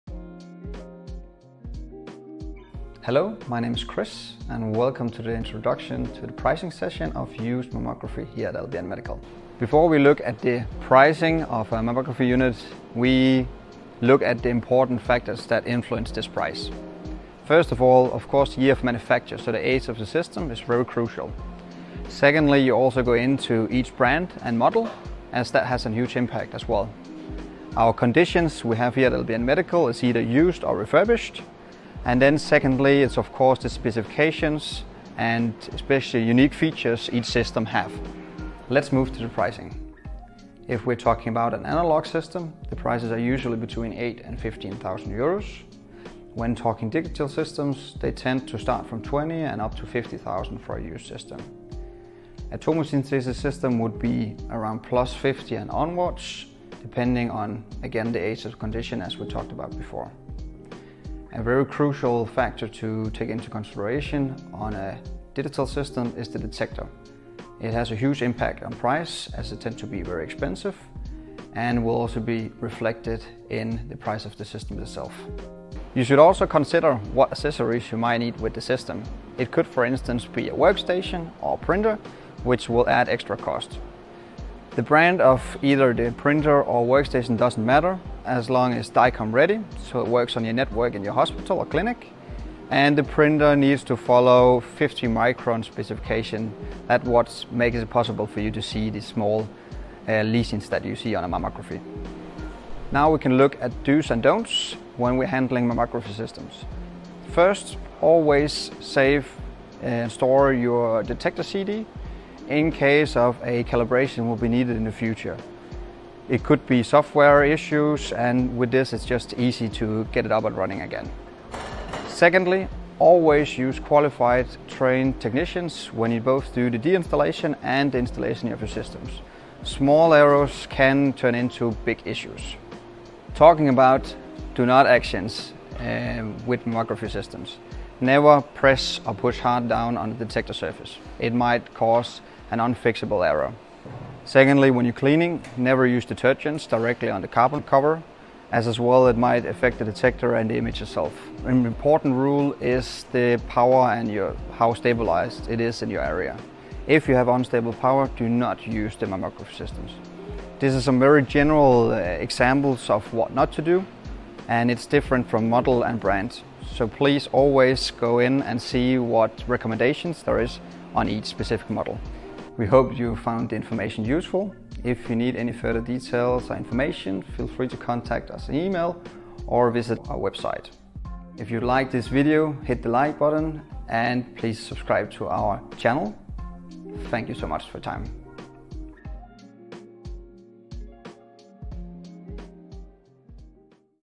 How Much Does a Mammography Machine Cost? - LBN Medical
The pricing of used medical imaging equipment can be complex.
In this video, our Business Unit Manager for Mammography, Kris Skaarup walks you through average pricing of mammography machines, and gives you useful tips on how to take care of your mammography machine.

Contents:
0:00 Intro to Pricing Session
0:09 Factors Affecting Pricing of mammo systems
0:39 What is the price of Analog mammography machine?
0:47 What is the price of Digital mammography machine?
0:54 How much does a Tomosynthesis mammography system cost?
1:17 Accessory recommendation
1:42 Mammo machine Do's and Don'ts
2:52 Outro

ANY QUESTIONS?